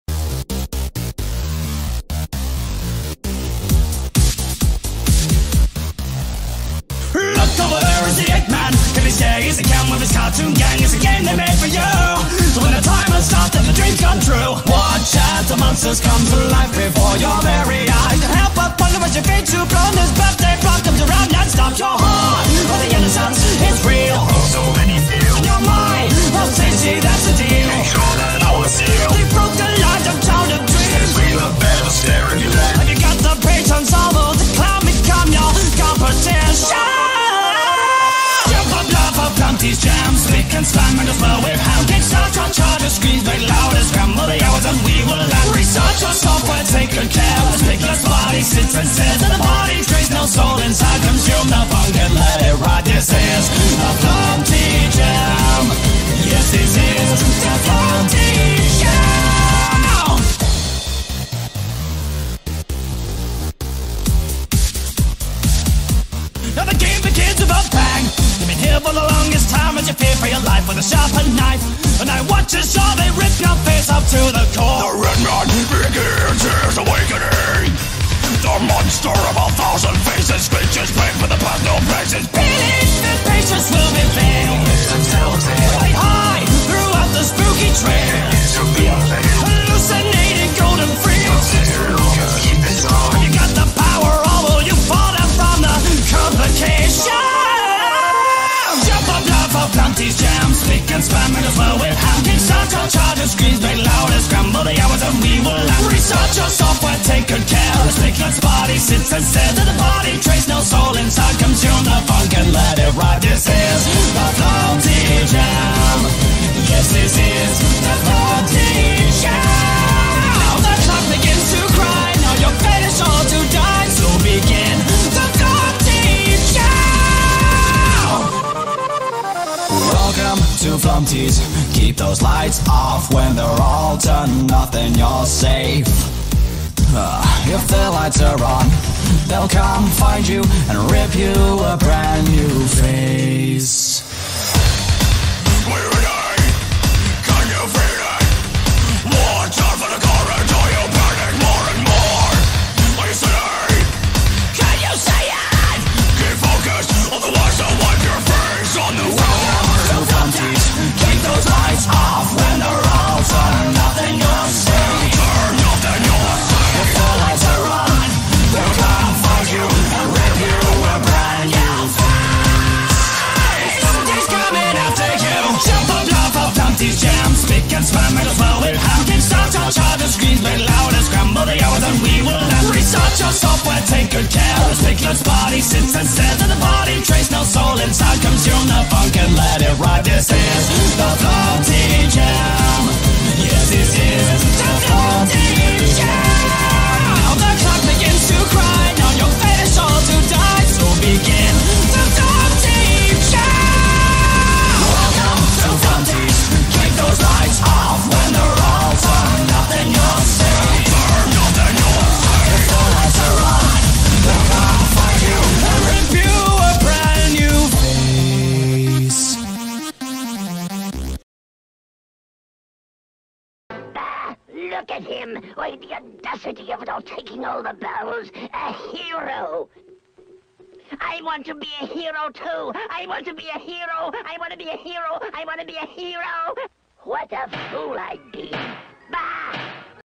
Professor Nutty-Nut Meg AMV Flumpty Jam
New video is up We have A Villain name Professor Nutty-Nut Meg who always attempt to steal Felix's magic bag